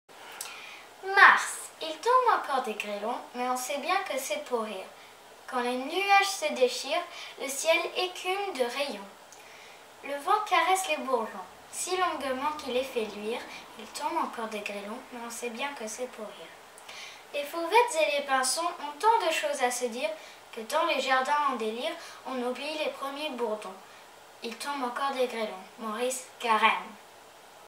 French Poem "Mars" de Maurice Carême - French Today
Mars, de Maurice Carême
Il tombe encore des grêlons,
Mais on sait bien que c'est pour rire.
Quand les nuages se déchirent,
Le ciel écume de rayons.

Le vent caresse les bourgeons
Si longuement qu'il les fait luire.

Les fauvettes et les pinsons
Ont tant de choses à se dire
Que dans les jardins en délire
On oublie les premiers bourdons.

Learn French like it is really spoken today with French Today audiobooks: level adapted audio novels recorded at several speeds and levels of enunciation.